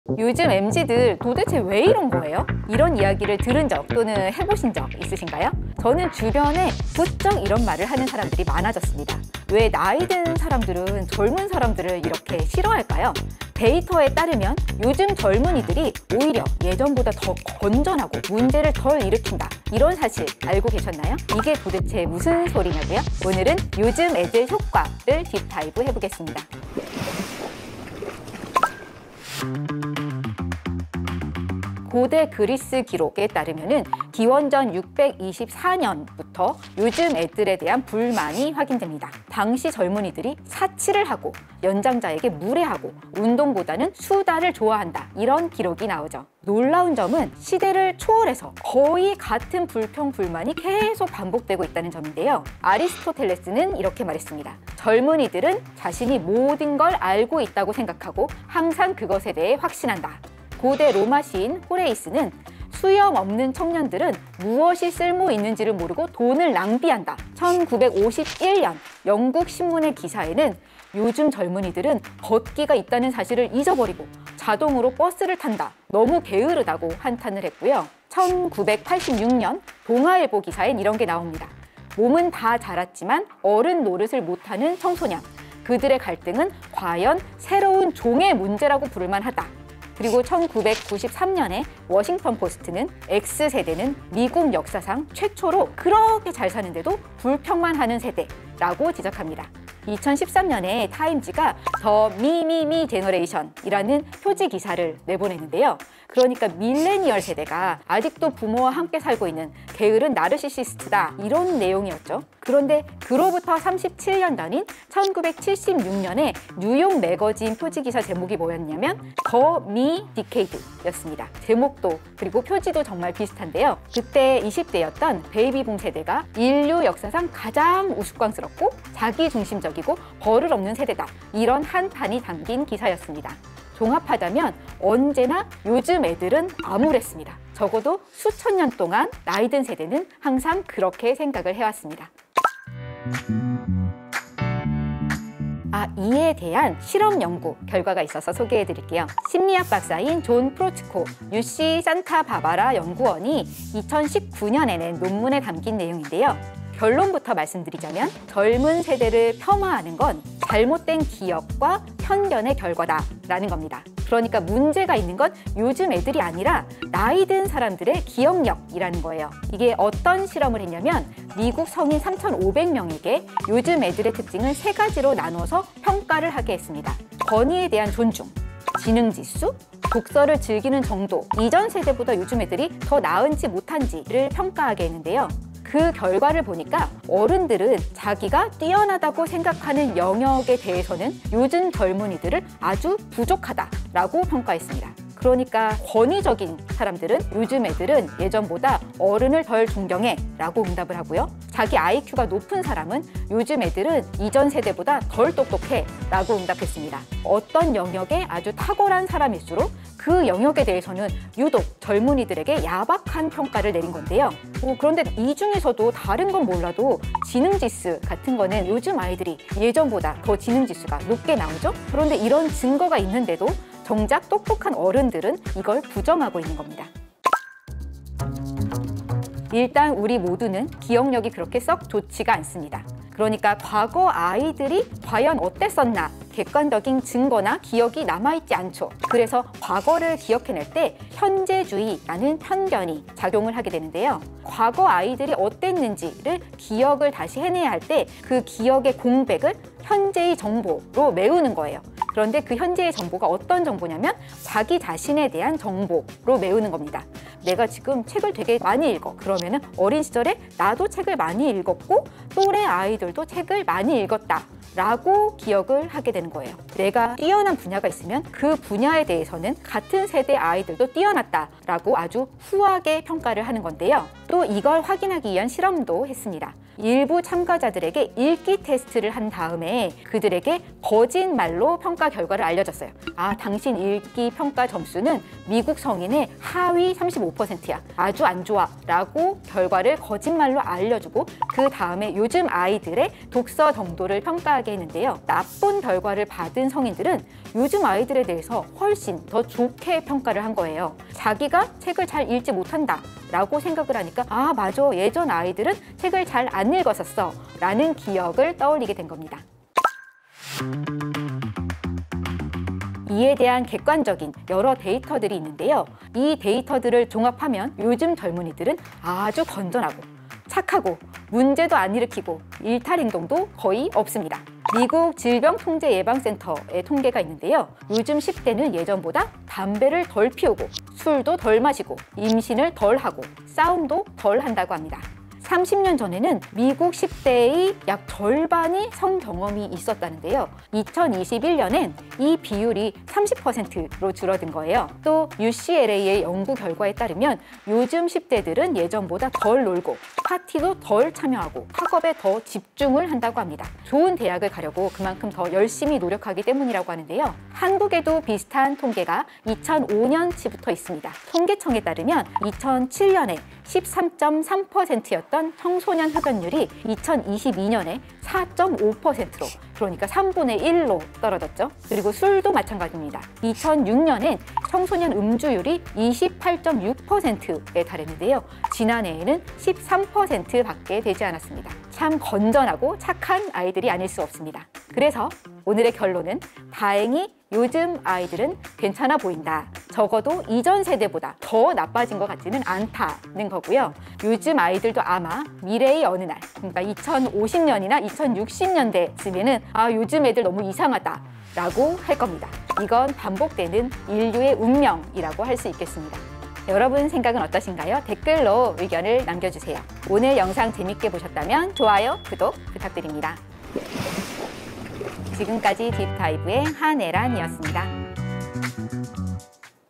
요즘 애들은 늘 어딘가 부족하답니다. 언제부터? 기원전 624년부터 ㅇㅇ [요즘 애들 효과] MZ, 세대 갈등, 혐오, 딥다이브
“요즘 애들은 도대체 왜 이래?” 이런 이야기 들은 적, 또는 해본 적 있나요? 왜 나이 든 사람들은 젊은이들을 그렇게도 미워할까요. 다 그럴 만한 이유가 있을 거라고요? 객관적으로 요즘 애들은 문제가 있다고요?

글쎄요. 심리학 연구 결과는 좀 다르게 나오는데요. 오늘은 ‘요즘 애들 효과(Kids these days effect)’를 들여다보겠습니다.

🌊재밌게 빠지는 경제상식! 렛츠 딥다이브🌊

00:00 오프닝
00:29 '요즘 애들' 이라는 말 기원전에도 있었다고요?
02:23 팩트로 봤을 때 쇠퇴하는 게 아니라고요?
03:48 편견이 작용한다고요? feat.기억력
05:10 객관적인 데이터에서 팩트는 뭔가요?

기획·구성 | 한애란
연출 | 신한솔 정승연
제작 | 신한솔 정승연 유니나
CP | 신준모
썸네일 l 정승연